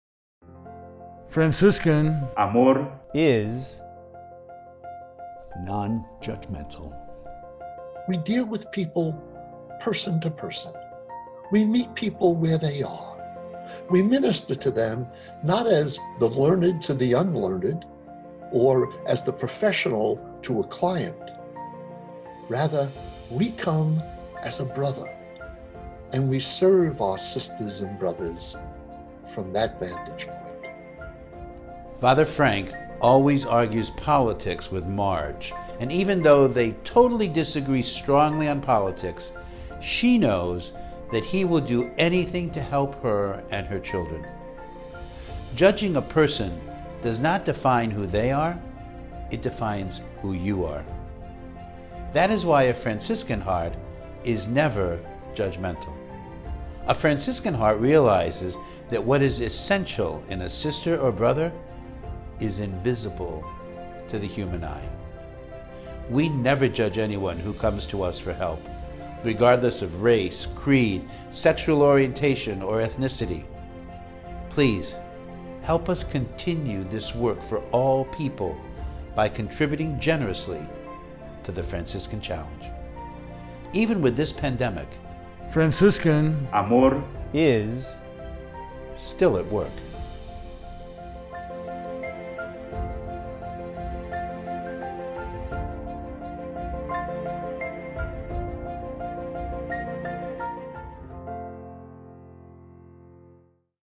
Franciscan Love Is Non-Judgmental: Take The Franciscan Challenge
The Franciscan Friars believe in living life with joy, seeing the good in everyone and accepting all people without judgment. We Friars never judge anyone who comes to us for help regardless of race, creed, sexual orientation, or ethnicity.

As COVID-19 and its impact continues, millions of people are experiencing painful losses, hardships and health challenges and they are coming to our doors for help.

Like many others, these people are dealing with unemployment, are struggling to buy food and may even be facing the possibility of becoming homeless.

We are urgently trying to help them as best we can, but it seems that more and more people are in desperate need.

The Franciscan Challenge is the Annual Fundraising Campaign for the Franciscan Friars – it is our major source of funding for our work.

Only when you take The Franciscan Challenge can we Friars continue to provide basic life necessities of food, shelter, and medical attention to our brothers and sisters in need. Remember, a non-judgmental heart realizes that what is essential in a sister or a brother is invisible to the eye.

Without YOU, our work will end. So far, we have provided groceries for over 4,550 families in the past 12 months. We have served 109,500 meals to the homeless who came to our door this past year, and they are still coming.

When you take the Franciscan Challenge, 100% of your donation goes to ministries providing services to the poor, assisting in the education of young Friars, and care for our elderly Friars who have cared for so many for so long. Only because of your help is our Franciscan Love Still At Work!